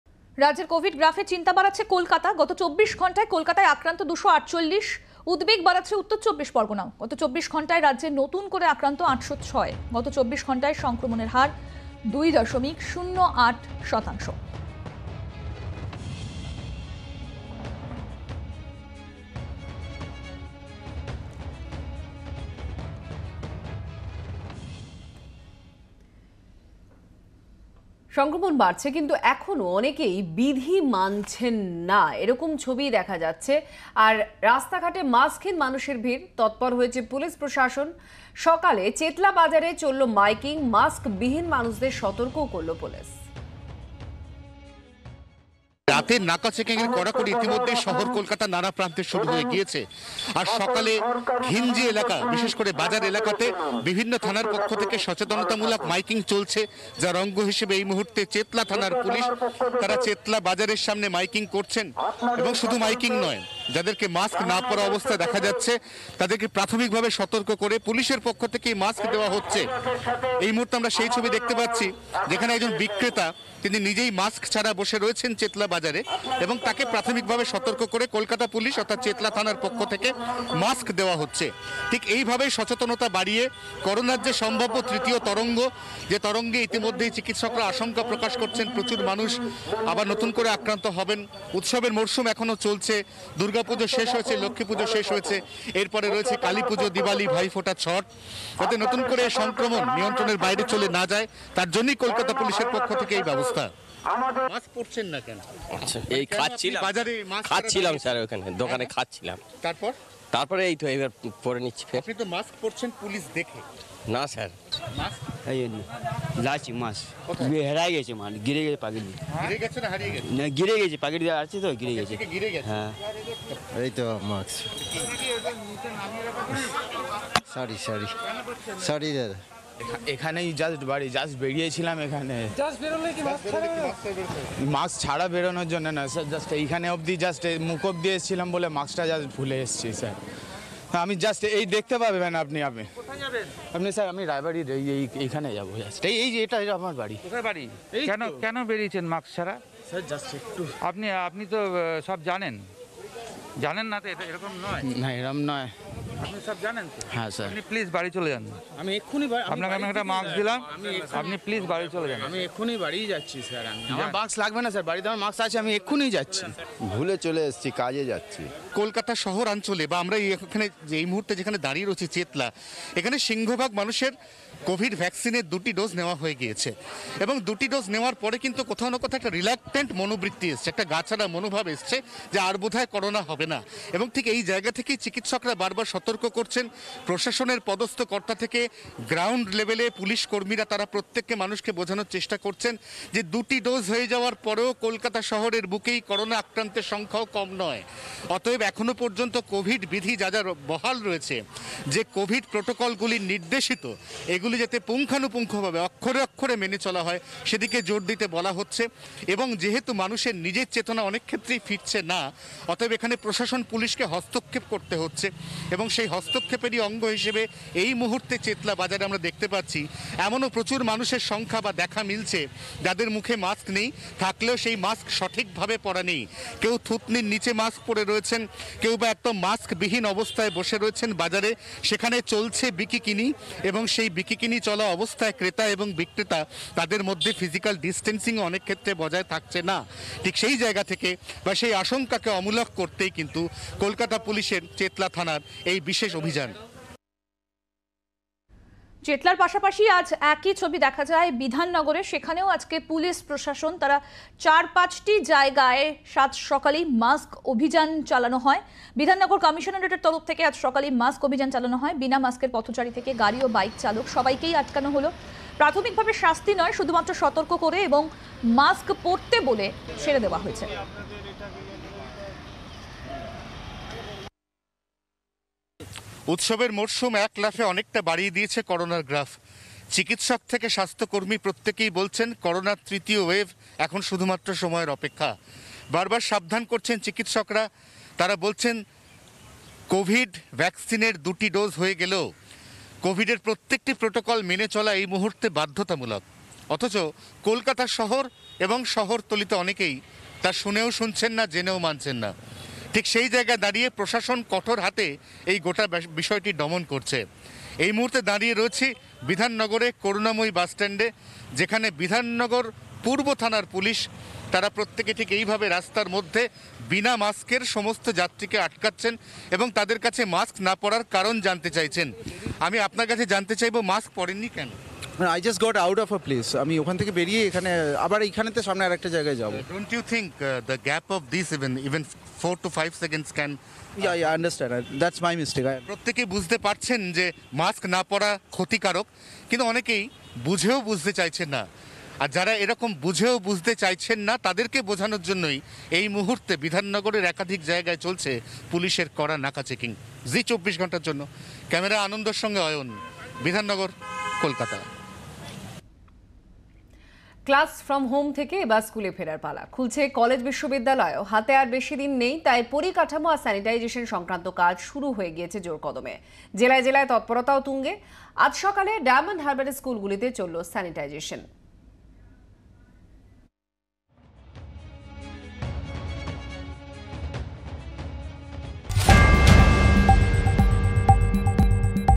Corona Alert: কলকাতার কোভিডগ্রাফ নিয়ে উদ্বেগ, রাজ্য সরকারকে চিঠি কেন্দ্রীয় স্বাস্থ্যসচিবের
কলকাতার কোভিডগ্রাফ নিয়ে উদ্বেগ, রাজ্য সরকারকে চিঠি কেন্দ্রীয় স্বাস্থ্যসচিবের